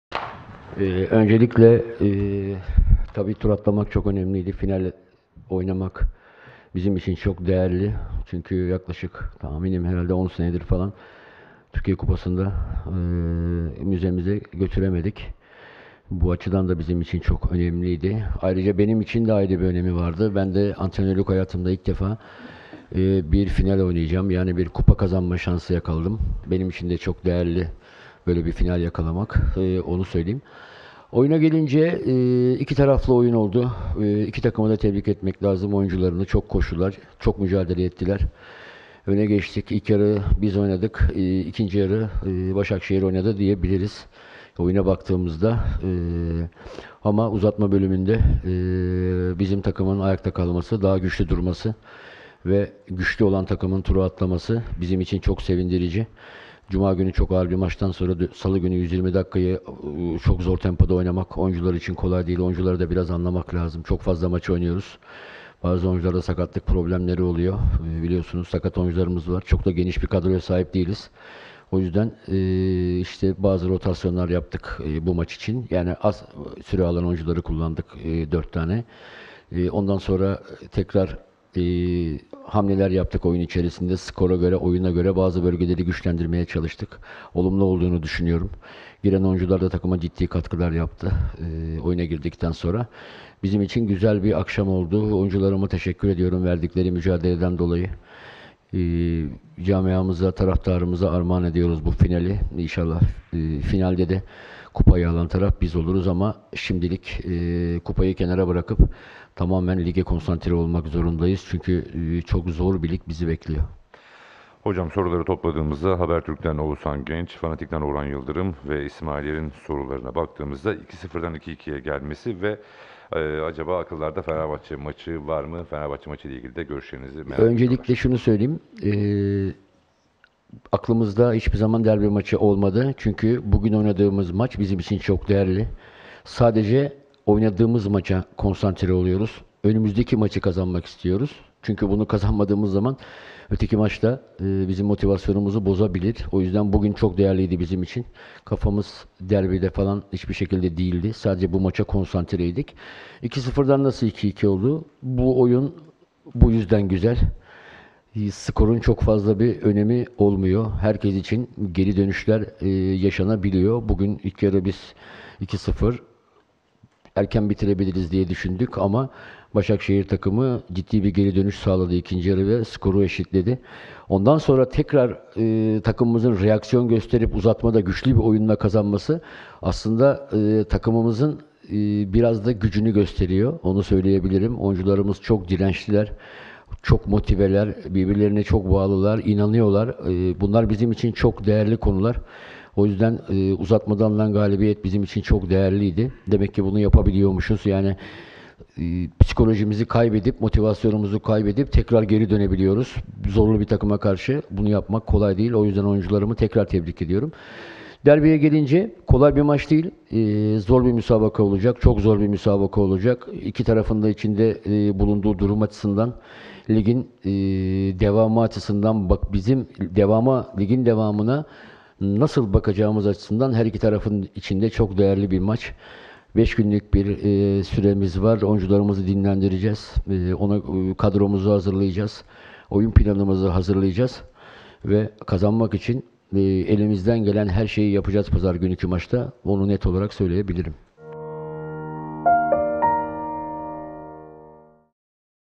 Sergen Yalçın'dan derbi açıklaması! "Kazanmak için..."
Beşiktaş, Ziraat Türkiye Kupası'nda finale yükseldi. Karşılaşma sonrası teknik direktör Sergen Yalçın açıklamalarda bulundu.